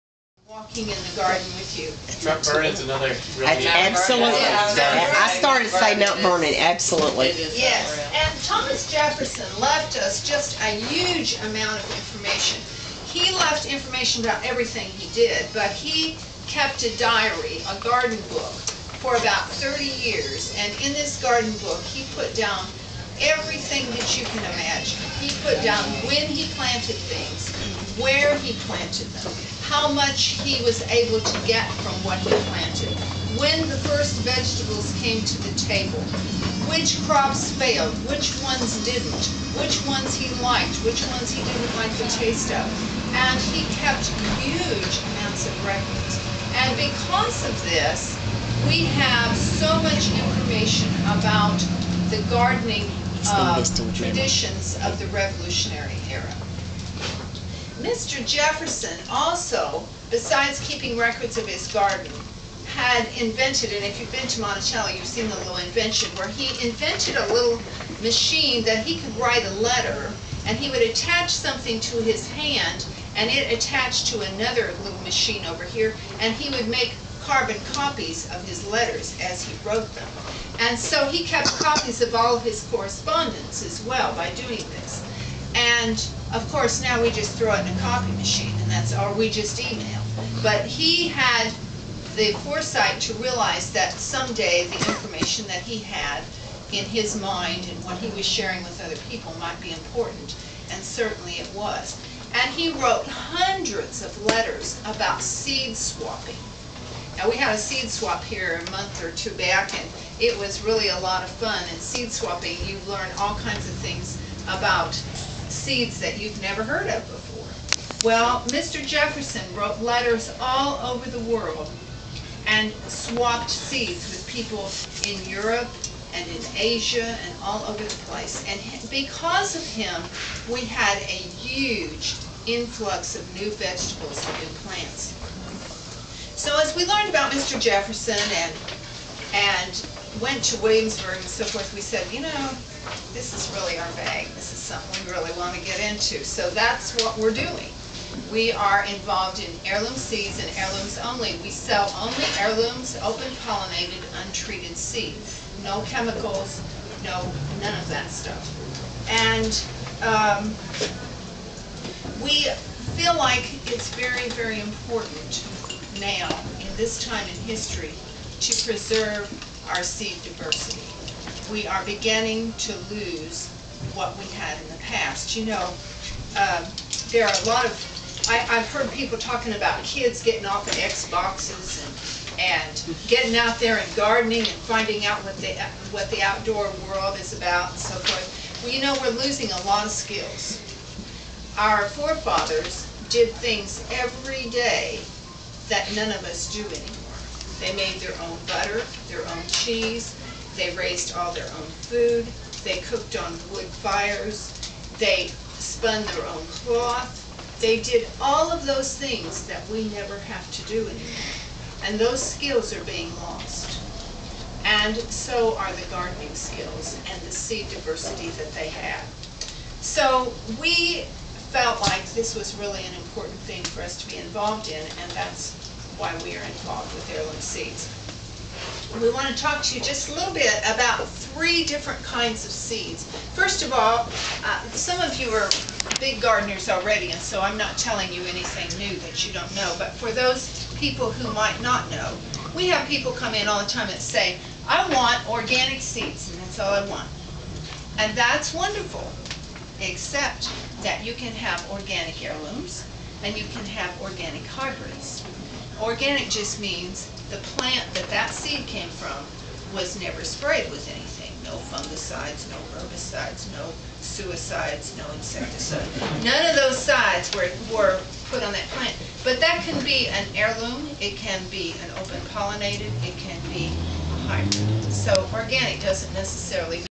Backyard Gardening 2013 S1 Opening
Discussion of Heirloom Seeds, Seed Diversity, Organic